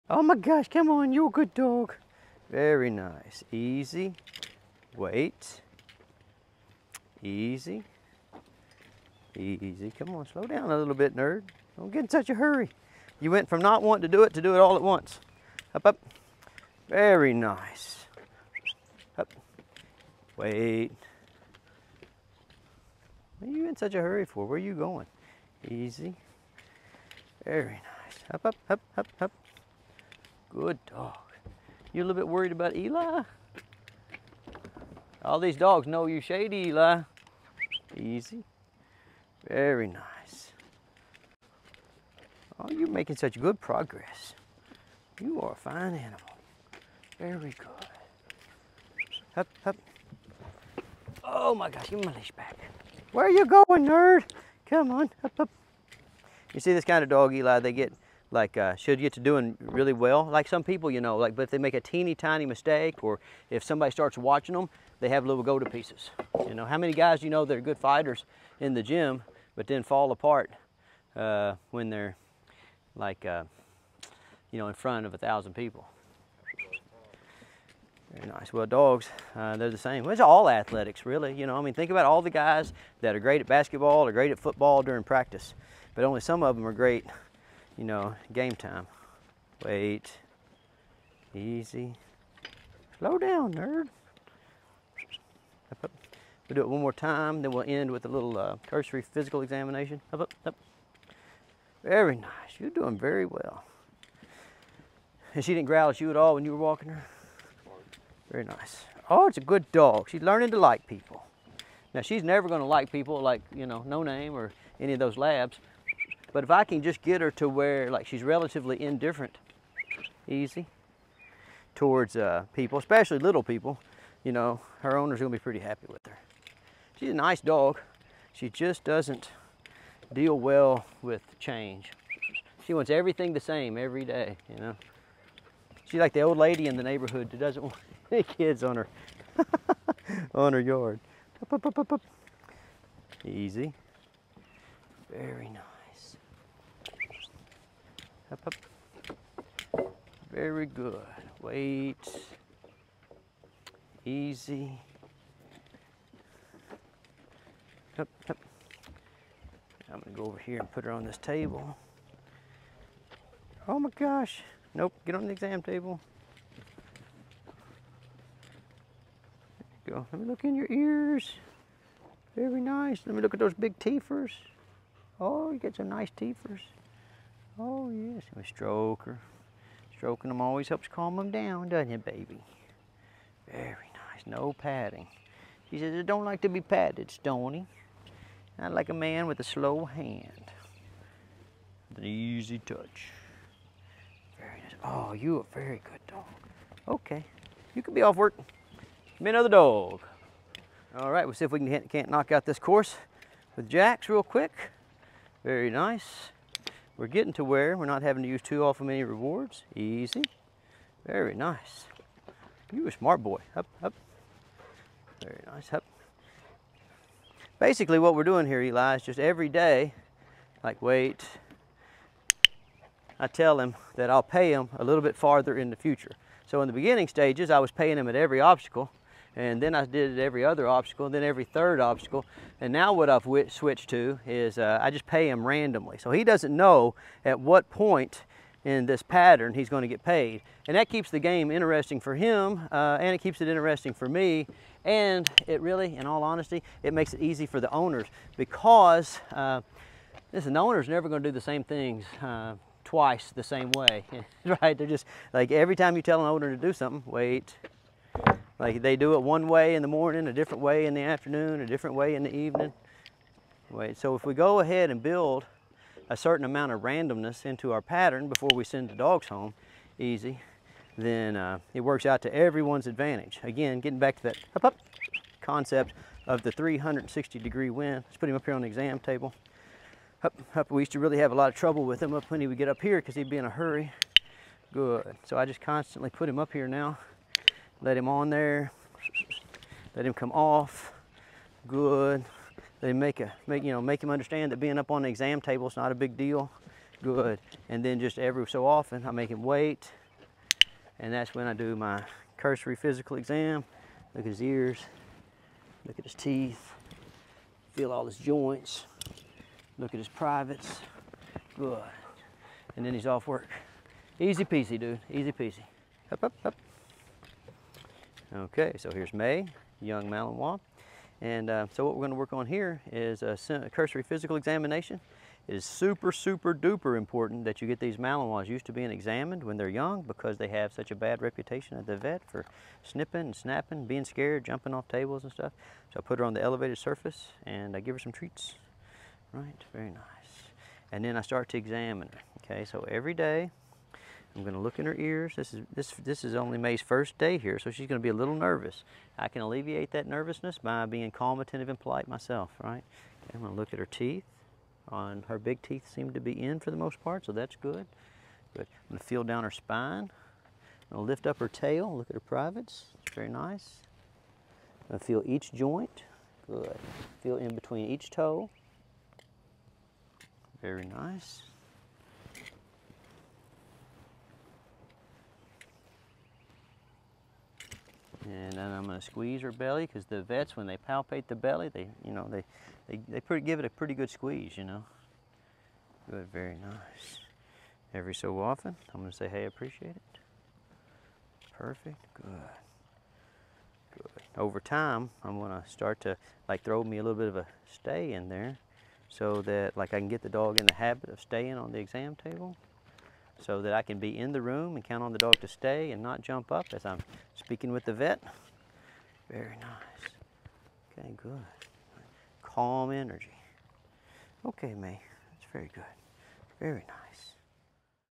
Behind The Scenes | Uncle Stonnie Demonstrates His Approach
We thought you guys might enjoy a behind the scenes look at what up and coming dog trainers see when they are working with Uncle Stonnie.

In this video we take a look at Uncle Stonnie training three different types of dogs and explaining to the observer the importance of technique selection, objective goal setting, and helping clients understand their dog's unique characteristics.

00:00 Shy Pit Bull Mix - Confidence Building
03:21 Labrador - Retrieve Remedial Training
05:16 Malinois Cursory Physical Examination